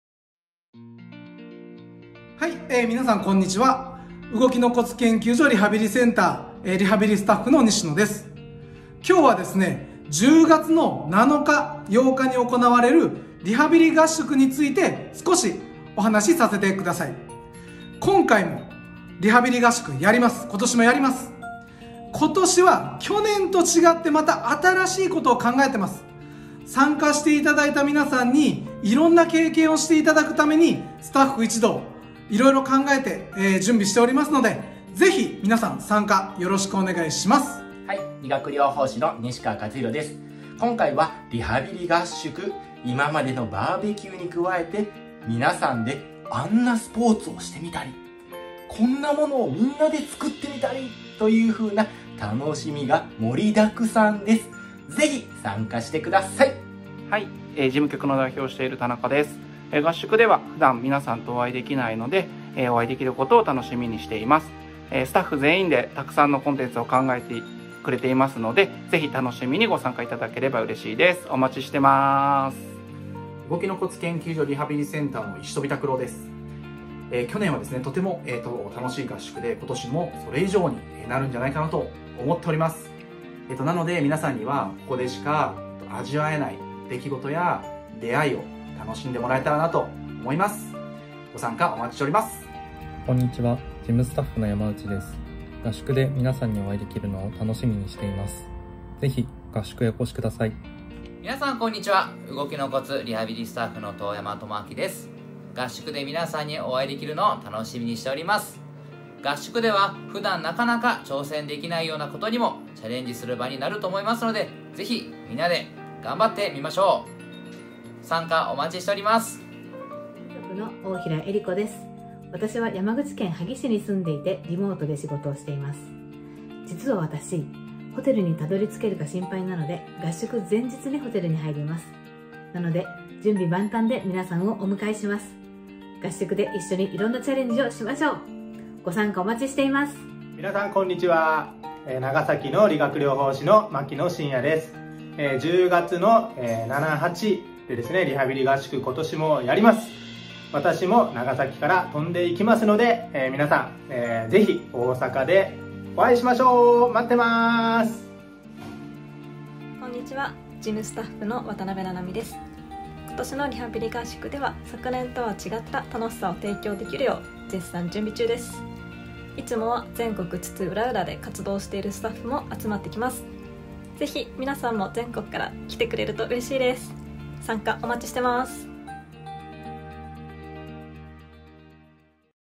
おいでよ！2024年リハビリ合宿✨
2024年のリハビリ合宿は、皆で一緒に楽しめるリハビリコンテンツが盛りだくさん！
参加申し込みは【8月11日（日）17時】まで！参加希望の方は以下のフォームからお申込みください✨

「知らない人が多くて不安」
「宿泊できるかな」
こんな不安をお持ちの方も、私たちがサポートしますので安心してください。
今回はそんなスタッフからの熱い想いをお届けします！！

【動きのコツ研究所リハビリセンター　脳梗塞・脳出血後遺症片麻痺リハビリ専門】
兵庫県西宮市池田町9-7フレンテ西館305号